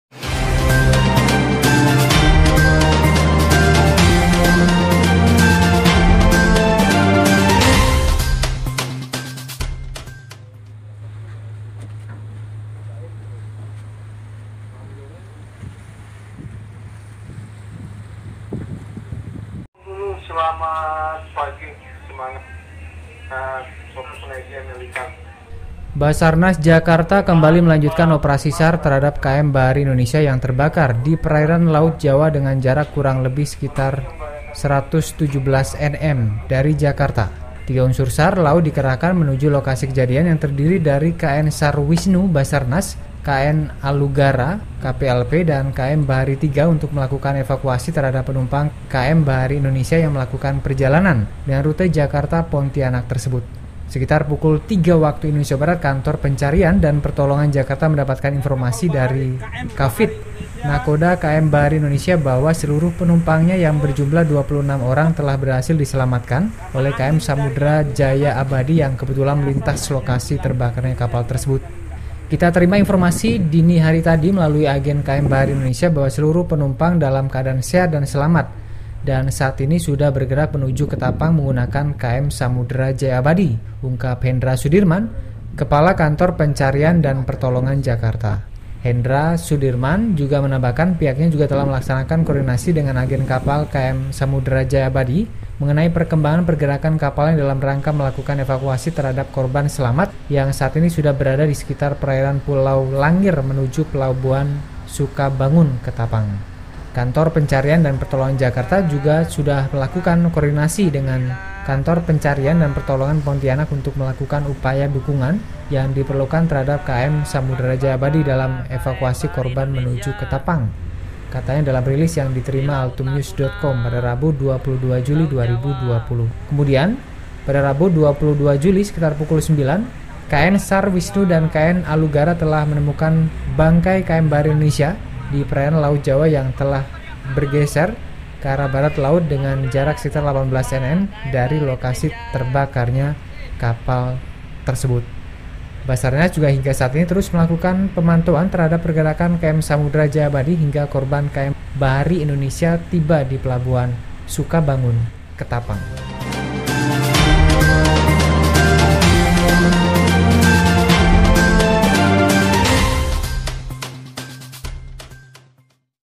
KM Bahari Indonesia Yang Terbakar di Laut Jawa Berhasil Dievakuasi, 26 Penumpang Selamat
ALTUMNEWS.com, JAKARTA -- Basarnas Jakarta kembali melanjutkan Operasi SAR terhadap KM Bahari Indonesia yang terbakar di Perairan Laut Jawa dengan jarak kurang lebih sekitar 117 Nm (Nauticalmiles) dari Jakarta.
Tiga unsur SAR Laut dikerahkan menuju lokasi kejadian yang terdiri dari KN SAR Wisnu (Basarnas), KN Alugara (KPLP), dan KM Bahari 3 untuk melakukan evakuasi terhadap penumpang KM Bahari Indonesia yang melakukan perjalan dengan rute Jakarta-Pontianak tersebut.
Sekitar pukul 03.00 WIB Kantor Pencarian dan Pertolongan Jakarta mendapatkan informasi dari Khafid (Nakhoda KM Bahari Indonesia) bahwa seluruh penumpangnya yang berjumlah 26 orang telah berhasil diselamatkan oleh KM Samudera Jaya Abadi yang kebetulan melintasi lokasi terbakarnya kapal tersebut.
"Kita terima informasi dini hari tadi melalui agen KM Bahari Indonesia bahwa seluruh penumpang dalam keadaan sehat dan selamat dan saat ini sudah bergerak menuju Ketapang menggunakan KM Samudera Jaya Abadi," ungkap Hendra Sudirman, Kepala Kantor Pencarian dan Pertolongan Jakarta selaku SMC (SAR Mission Coordinator) dalam operasi SAR.
Hendra Sudirman juga menambahkan pihaknya juga telah melakukan koordinasi dengan agen Kapal KM Samudera Jaya Abadi mengenai perkembangan pergerakan kapalnya dalam rangka melakukan evakuasi terhadap korban selamat yang saat ini sudah berada di sekitar Perairan Pulau Langir menuju Pelabuhan Sukabangun Ketapang.
“Kantor Pencarian dan Pertolongan Jakarta juga sudah melakukan koordinasi dengan Kantor Pencarian dan Pertolongan Pontianak untuk melakukan upaya dukungan yang diperlukan terhadap KM Samudera Jaya Abadi dalam evakuasi korban menuju Ketapang,” katanya dalam release yang diterima Altumnews.com, Rabu (22/07/2020).
Kemudian pada Rabu (22/7) sekitar pukul 09.00 WIB pagi tadi KN SAR Wisnu dan KN Alugara telah menemukan bangkai KM Bahari Indonesia di Perairan Laut Jawa yang telah bergeser ke arah barat laut dengan jarak sekitar 18 Nm dari lokasi terbakarnya kapal tersebut.
"Kondisi kapal masih terapung dengan api sudah padam kemudian kita lakukan penyisiran pada kapal tersebut dan didapat hasil sudah tidak ada korban lagi yang dievakuasi," tambah Hendra.
Basarnas juga hingga saat ini terus melakukan pemantauan terhadap pergerakan KM Samudera Jaya Abadi hingga korban KM Bahari Indonesia tiba di Pelabuhan Sukabangun Ketapang.***
Editor : Robert